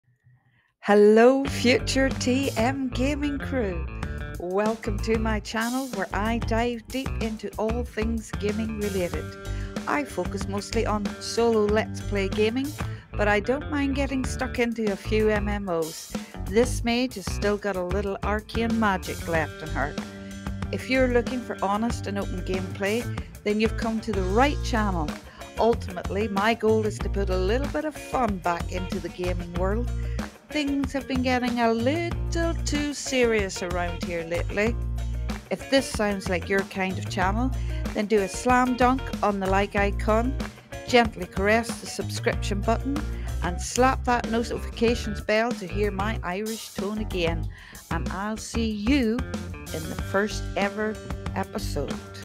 Tahane Mage Gaming - Welcome to the channel
A quick welcome message to the channel from me to you.